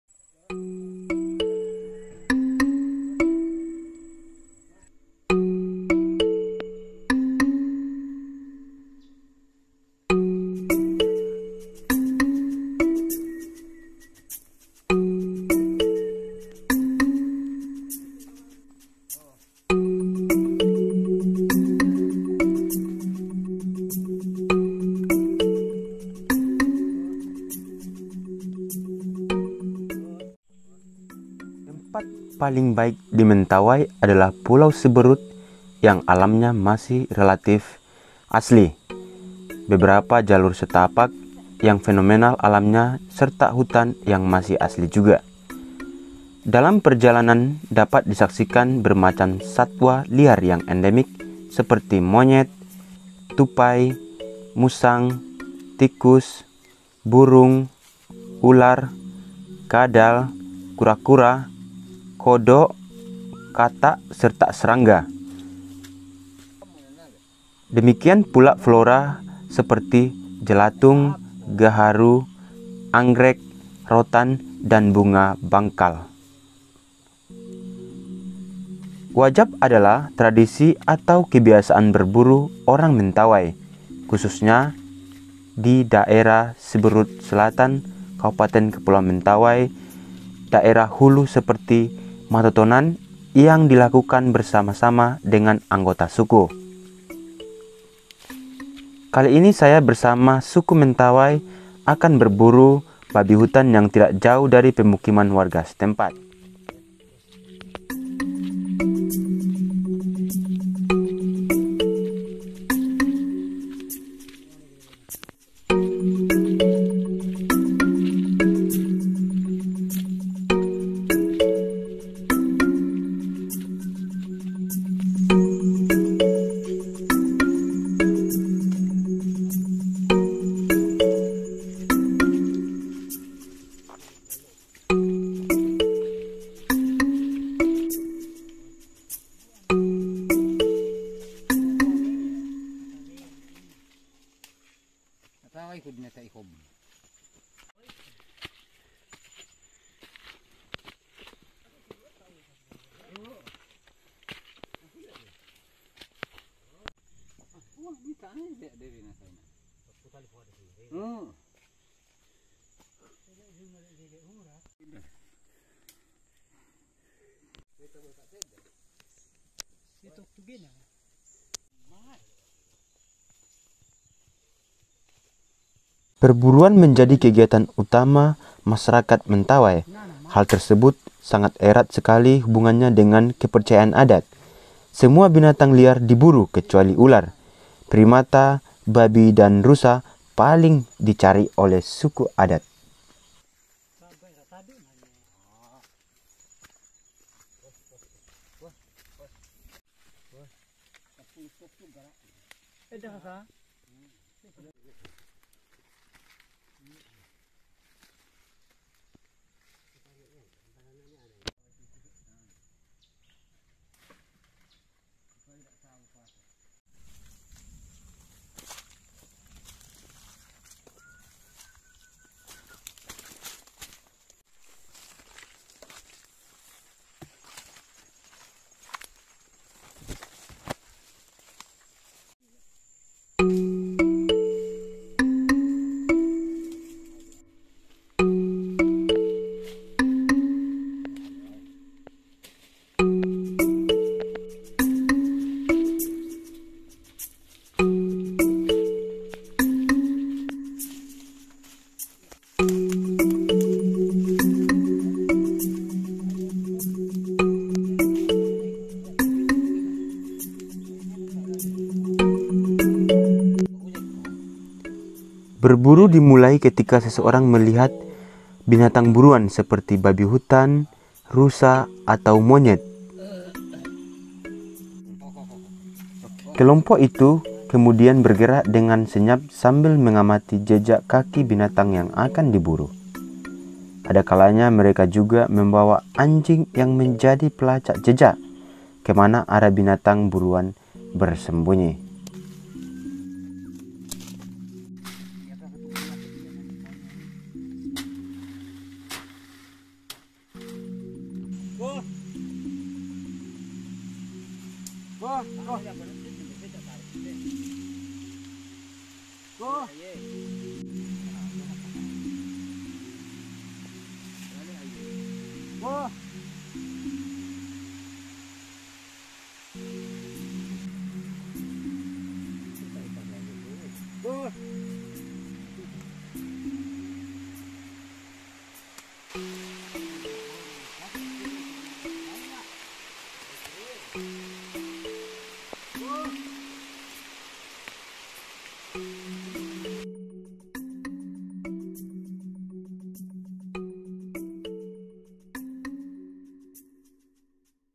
Berburu Babi Liar, Bersama Anjing Kampung Pemburu@Paulusmentawai
Halo guys, kali ini membahas tentang berburu babi liar, dengan anjing kampung pemburu. kebiasaan ini merupakan tradisi orang suku mentawai yang sudah menjadi kebiasaan atau proses pesta adat. Ini kegiatan berburu sangat sakral yang dilakukan oleh suku mentawai@Paulusmentawai